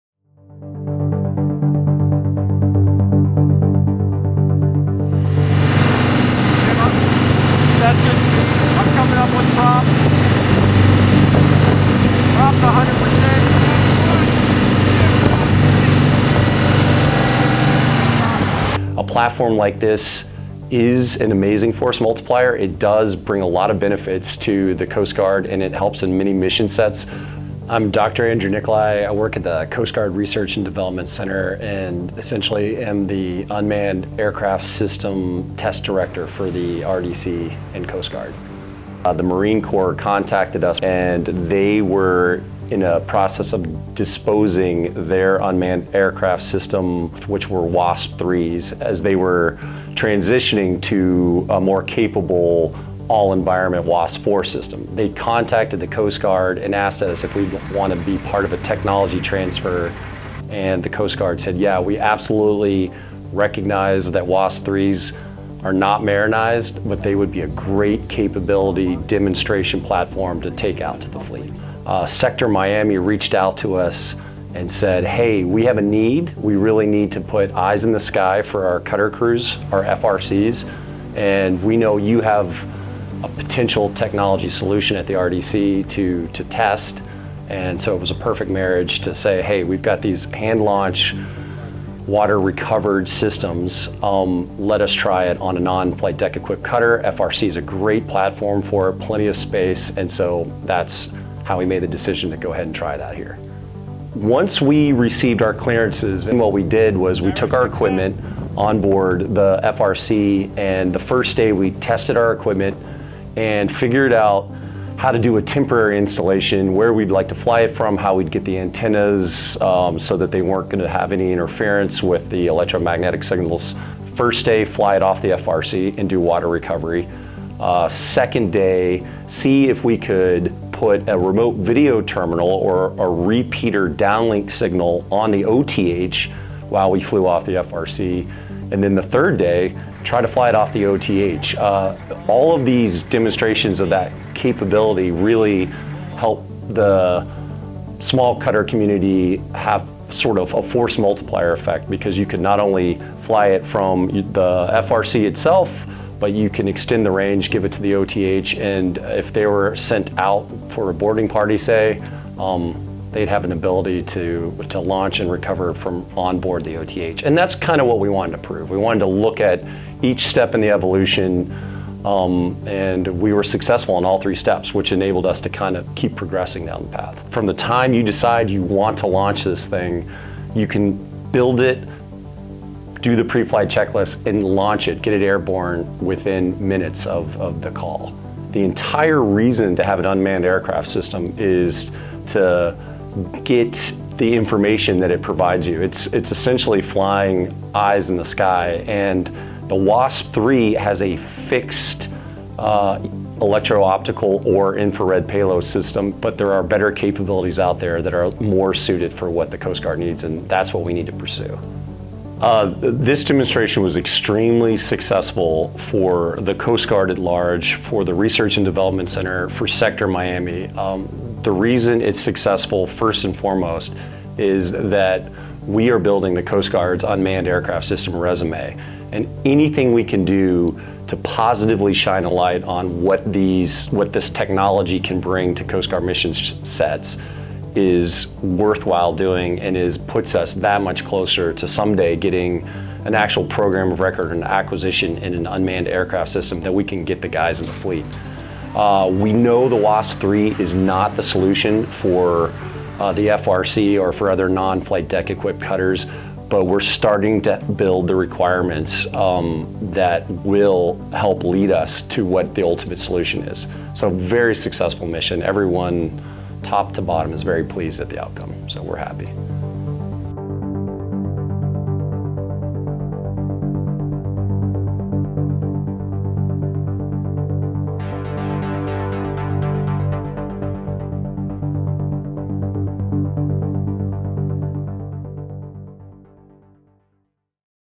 Coast Guard Wasp III UAS Demonstration
Dr. Andrew Niccolai, with the Coast Guard Research and Development Center, talks about the recent Wasp III unmanned aerial vehicle demonstration and tests near Miami Beach. The RDC has been testing multiple UAS platforms in the search for an acquisition. U.S. Coast Guard video by Petty Officer 2nd Class Luke Clayton.